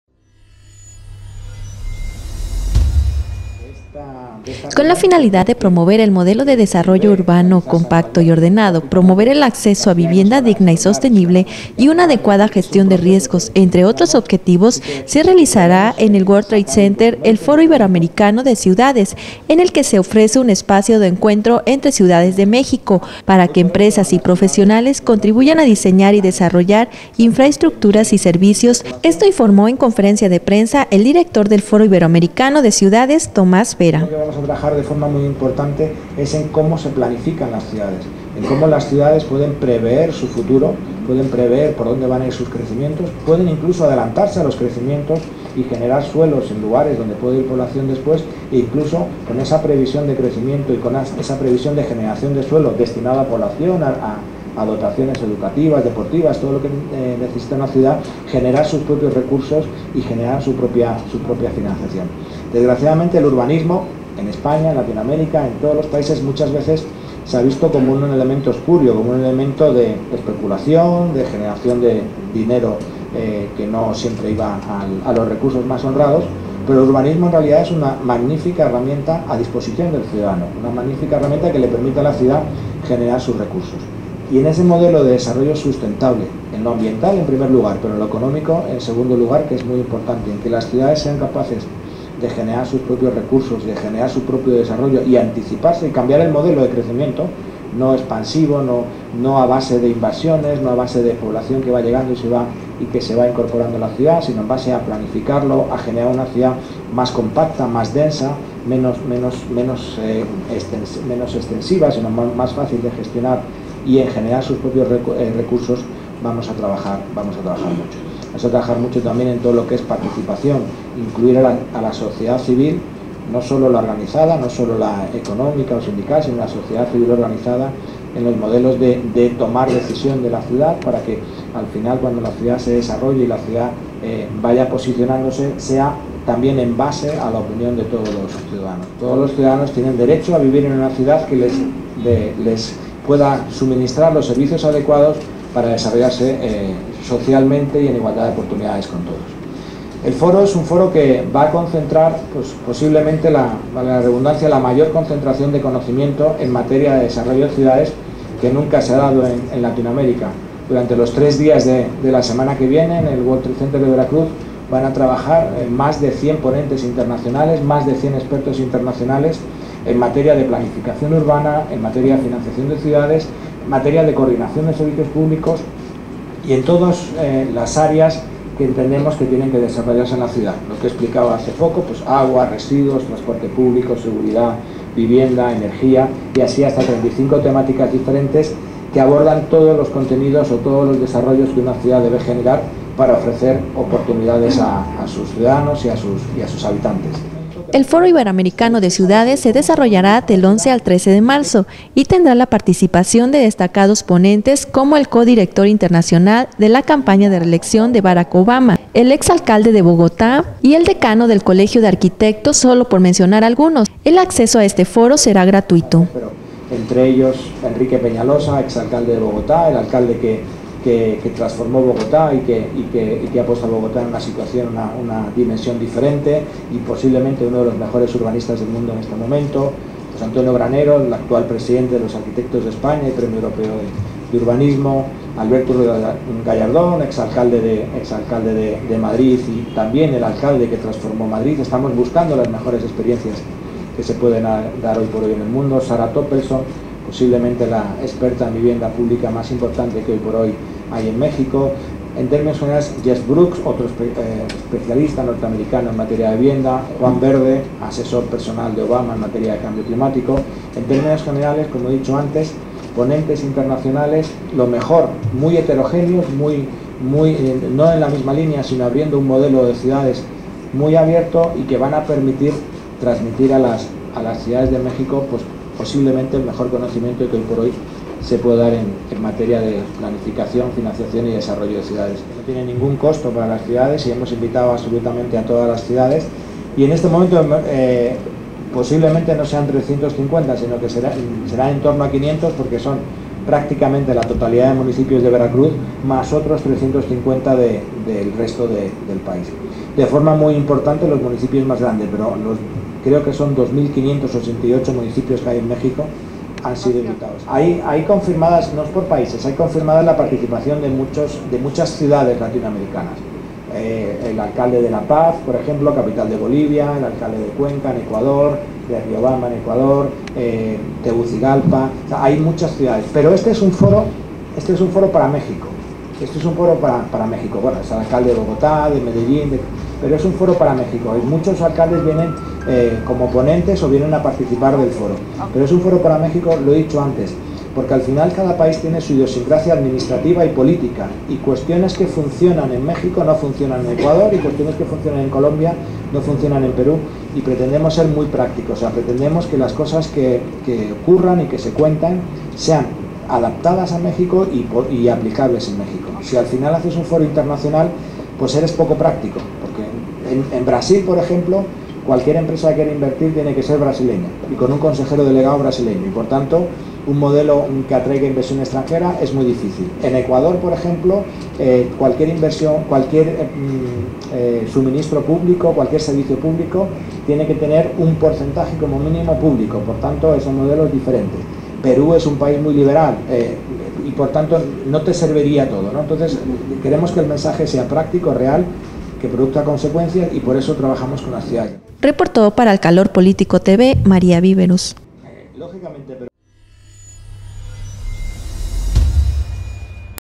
Para mejorar las ciudades
Xalapa, Ver., 5 de marzo de 2015.- En conferencia de prensa, Tomás Vera Romeo, dio a conocer detalles del "Foro Iberoamericano de Ciudades”, que se desarrollará en el World Trade Center de Boca del Río, del 11 al 13 de marzo.